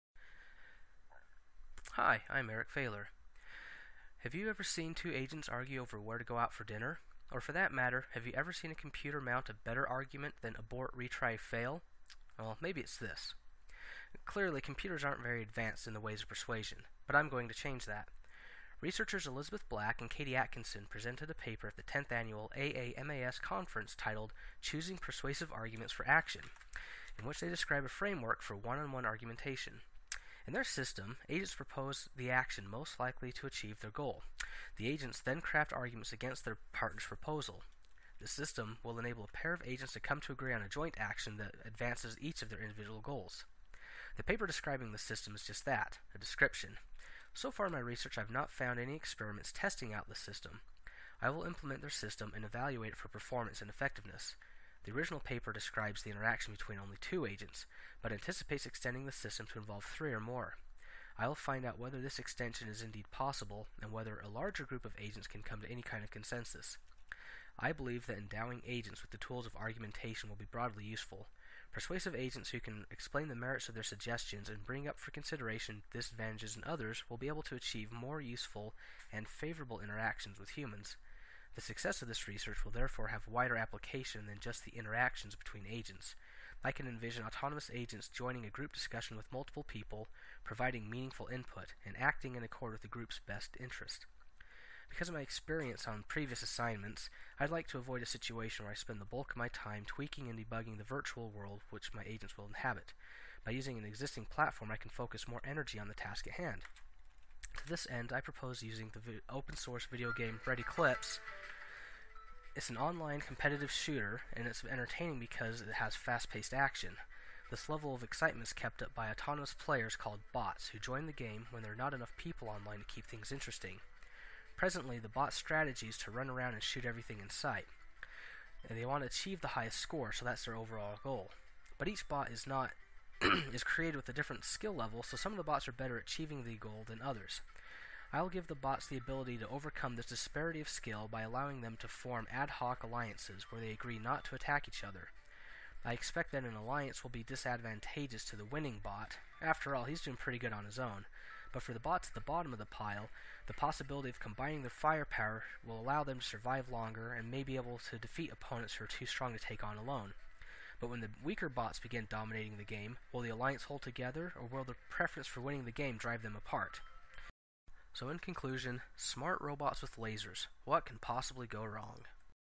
Erik Falor's CS6100 Project Elevator Speech
In which I propose to implement an argumentation system where autonomous agents can debate the relative merits of each others' propositions.  The agents involved will come to agree on a single course of action to take together.